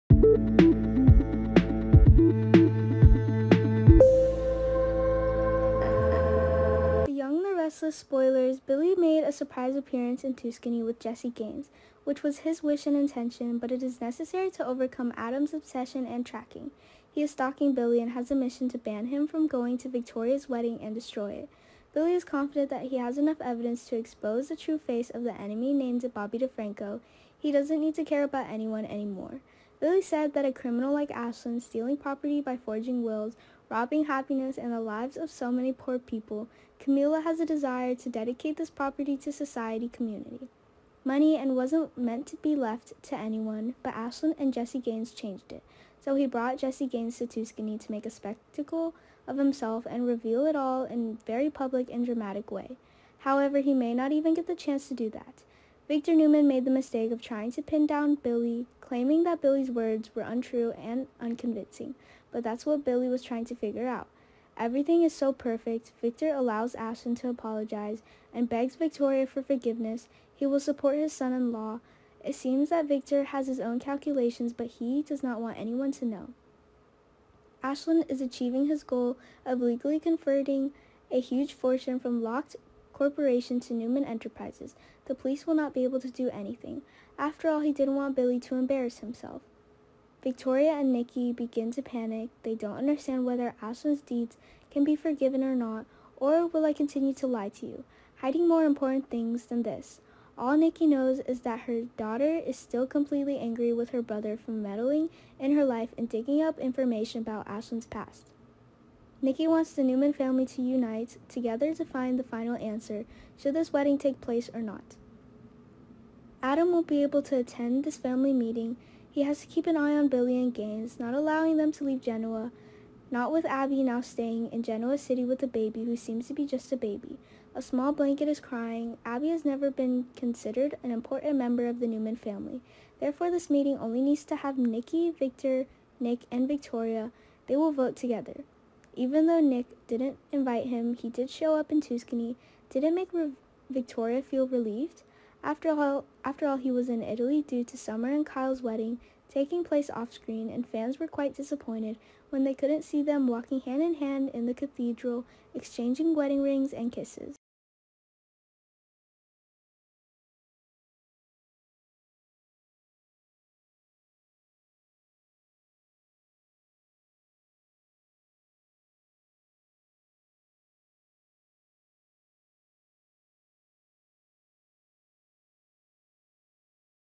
The Young and the Restless Spoilers Next Weeks Victor threaten Billy to stay away from Victoria
The Young and the Restless (Y&R) Spoilers Billy made a surprise appearance in Tuscany with Jesse Gaines, which was his wish and intention. But it is necessary to overcome Adam's obsession and tracking, he is stalking Billy and has a mission to ban him from going to Victoria's wedding and destroy it.

Billy is confident that he has enough evidence to expose the true face of the enemy named Bobby Defranco, he doesn't need to care about anyone anymore.

Billy said that a criminal like Ashland, stealing property by forging wills, robbing happiness and the lives of so many poor people, Camilla has a desire to dedicate this property to society, community. money and wasn't meant to be left to anyone, but Ashland and Jesse Gaines changed it. So he brought Jesse Gaines to Tuscany to make a spectacle of himself and reveal it all in a very public and dramatic way. However, he may not even get the chance to do that.

Victor Newman made the mistake of trying to pin down Billy, claiming that Billy's words were untrue and unconvincing, but that's what Billy was trying to figure out. Everything is so perfect. Victor allows Ashland to apologize and begs Victoria for forgiveness, he will support his son-in-law. It seems that Victor has his own calculations but he doesn't want anyone to know.